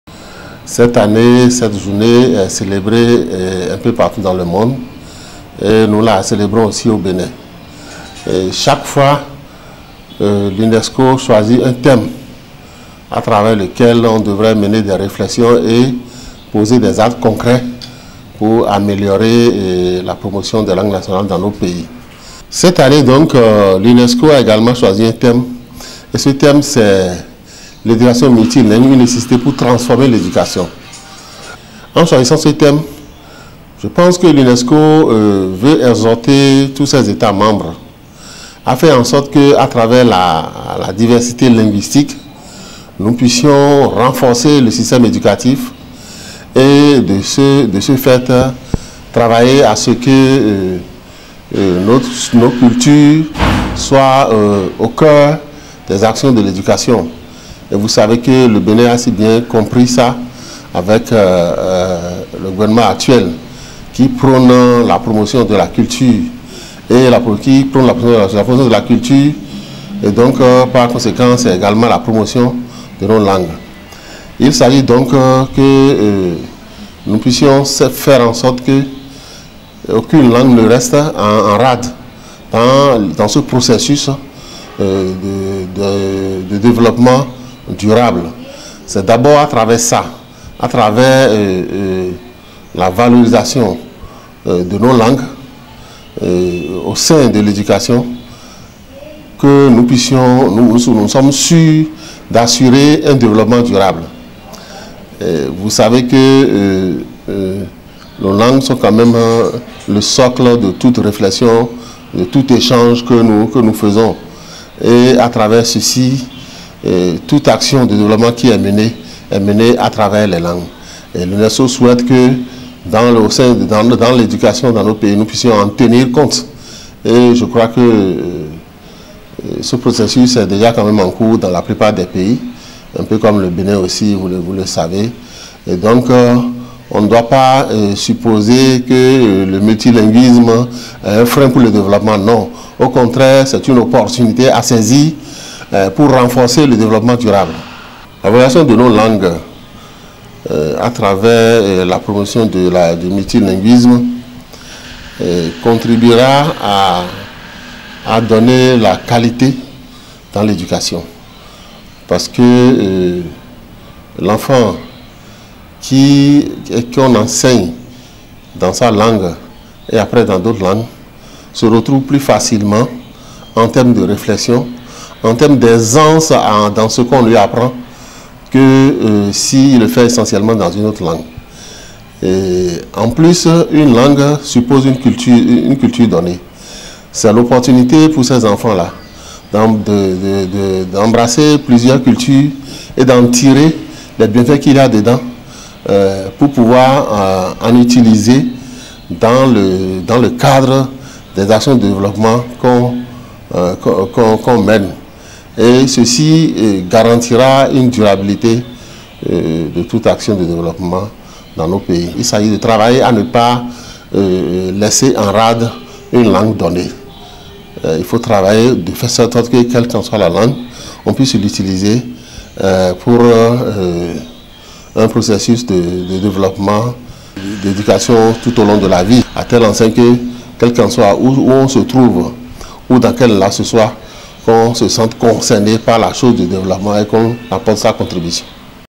Journée Internationale de la langue Maternelle au Bénin.
'' L'éducation multilingue, une nécessité pour transformer l'éducation'', c'est sous ce thème que le monde entier a célébré récemment la journée internationale de la langue maternelle.
Un thème au travers duquel l' UNESCO veut exhorter ses états membres, au renforcement du système éducatif à travers la diversité linguistique. C'est ce que pense Franck Arnold SEDJRO, secrétaire Exécutif du Réseau  National des  opérateurs Privés  pour la  Promotion de l'Alphabétisation  et  des Langues (RENOPAL). Au Bénin, le gouvernement actuel a bien compris la chose avec une autre manche de la promotion culturelle depuis son avènement, rappelle-t-il. Franck SEDJRO conseille d'agir afin qu'aucune langue ne soit oublié dans le processus de développement durable. Car, ajoute-t-il, le multilingue est une opportunité de renforcement du développement durable. Suivons Franck Arnold SEDJRO du RENOPAL